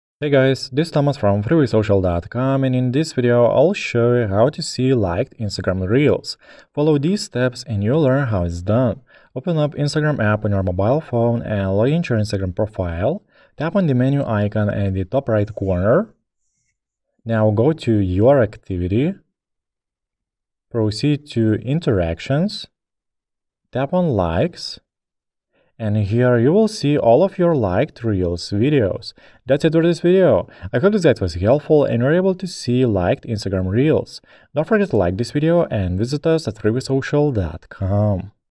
How To See Liked Instagram Reels
In this video I am going to show you how to see liked Instagram Reels.

Follow these steps and you will learn how it’s done:

1. Open up Instagram app on your mobile phone and login to your Instagram profile.
2. Tap on the menu icon at the top right corner.
3. Now go to “Your activity”.
4. Proceed to “Interactions”.
5. Tap on “Likes” and here you will see all of your liked Reels videos.

I hope this guide was helpful and you were able to see liked Instagram Reels. Don’t forget to like this video and visit us